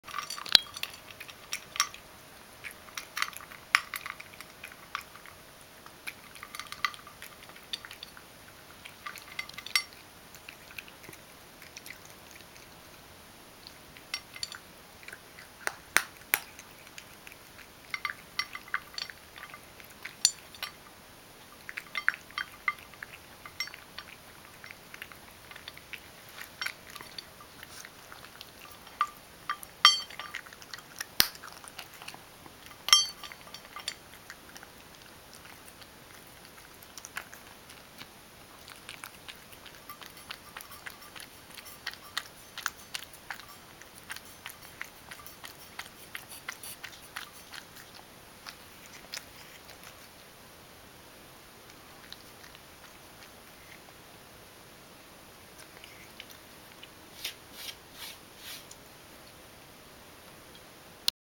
【猫のもじお君（ラグドール）】 ごはんカリカリ 【Cat (ragdoll) Food delicious】
餌をよく噛んでいい音を出して食べています。とてもおいしそうに食べている姿もかわいいです。最後は完食してほりほりしていました。
Chewing well the food we eat out a good sound. Figure you are eating very appetizing to be cute.
咀嚼以及我们吃了好声音的食物。图你吃很开胃是可爱。